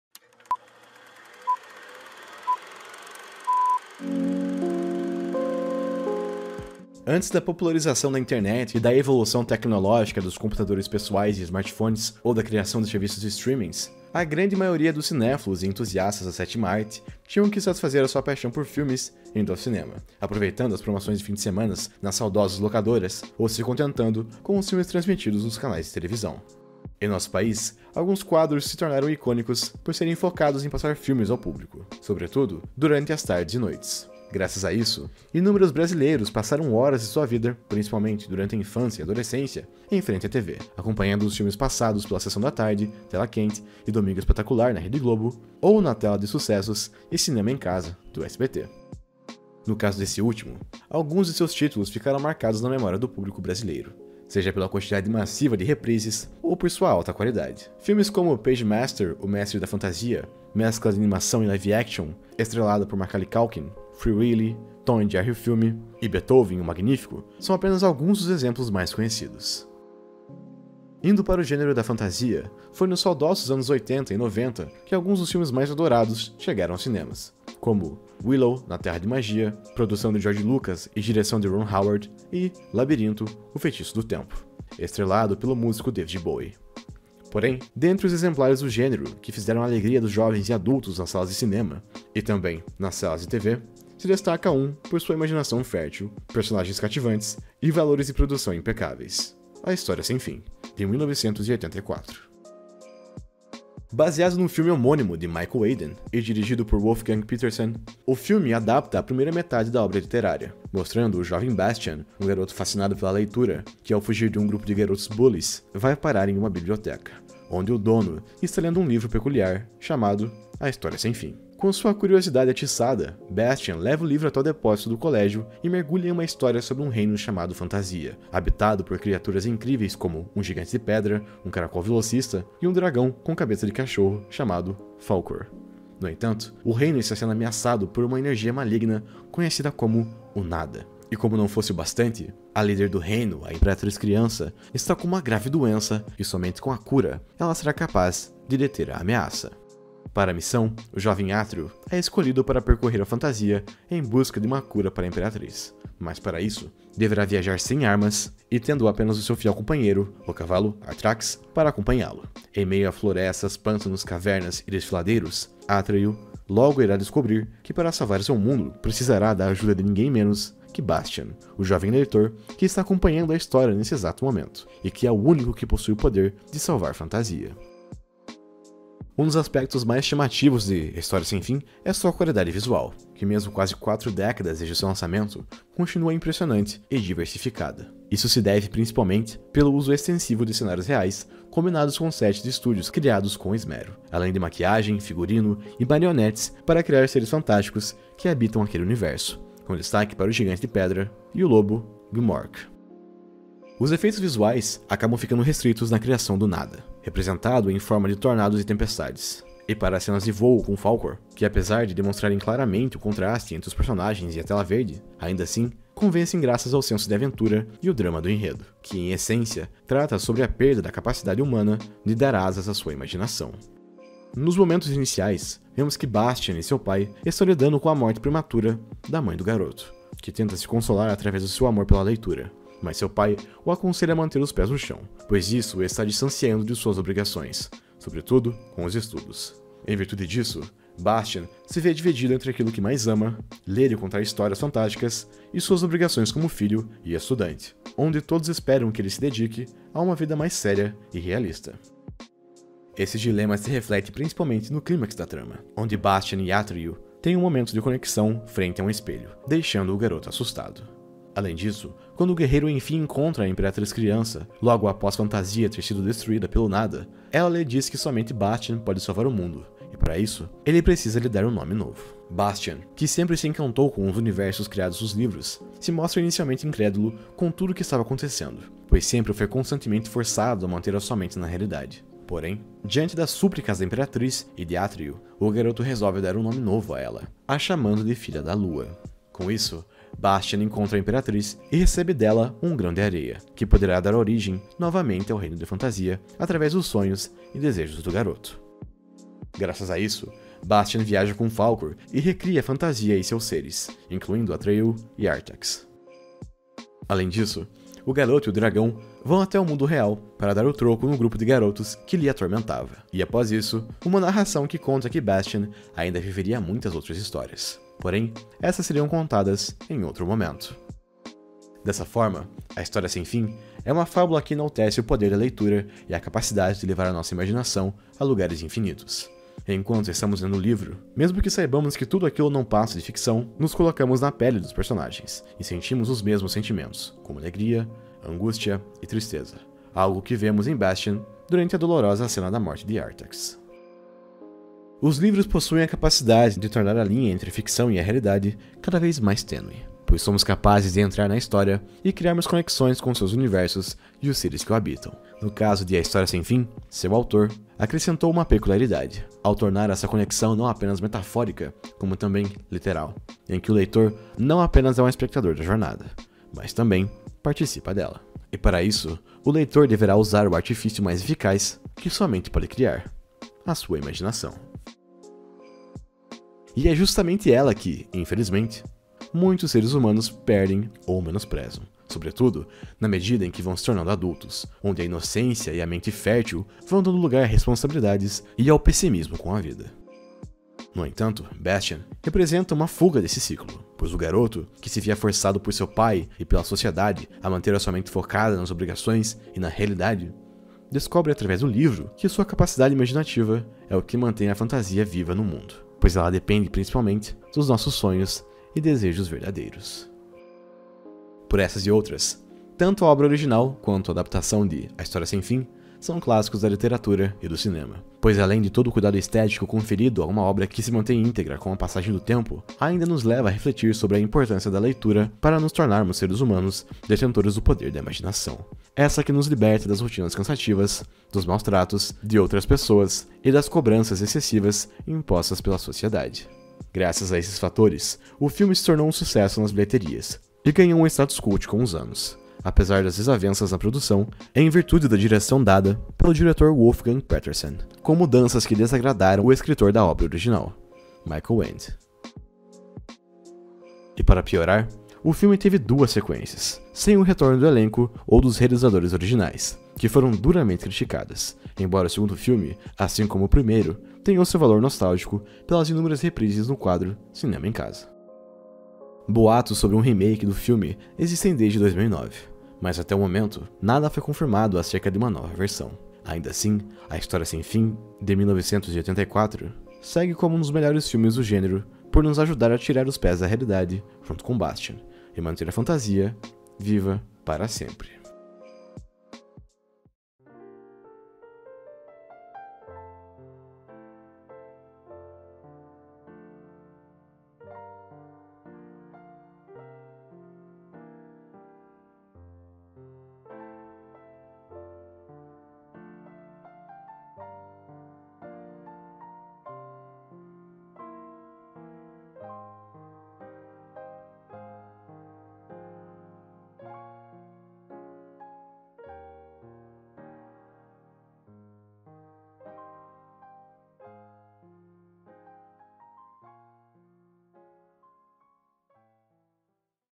"A História Sem Fim": dar asas à imaginação mantém a fantasia viva
Filmes, séries, desenhos e muito mais para assistir no UOL Play. Confira
Análise sobre o filme "A História Sem Fim" (1984), dirigido por Wolfgang Petersen.
Embora os principais representantes atuais da fantasia tradicional no cinema sejam as aclamadas franquias "Harry Potter" e "O Senhor dos Anéis", ambas surgidas no início do século XXI, os anos 80 e 90 foram responsáveis por alguns dos títulos mais adorados e nostálgicos que o gênero concebeu, como "Willow - Na Terra da Magia", "Labirinto - A Magia do Tempo", "A Princesa Prometida", "O Feitiço de Áquila", e tantos outros. Curiosamente, a grande maioria deles fez parte da infância e adolescência de muitos brasileiros, que antes da internet e dos smartphones, conferiam filmes através de quadros famosos da TV como Sessão da Tarde e o Cinema em Casa. No caso deste último, um filme seria reprisado inúmeras vezes e que, assim como no cinema, encantou públicos de todas as idades pela televisão com sua diversidade temática, personagens cativantes e valores de produção que seguem impressionantes até mesmo após quase 40 anos desde o seu lançamento: "A História Sem Fim". Adaptação do livro homônimo de Michael Ende e dirigido por Wolfgang Petersen, o filme transporta a primeira metade da obra literária, mas graças ao roteiro bem escrito e o espetáculo visual e sonoro, este é um dos melhores exemplares do gênero por, dentre outras coisas, mostrar através da paixão pela leitura do pequeno Bastian e da coragem do jovem guerreiro Atreyu a importância da imaginação para manter a fantasia sempre viva.

Roteiro: Bruno Cavalcanti
Edição/Locução: Alexandre Damaso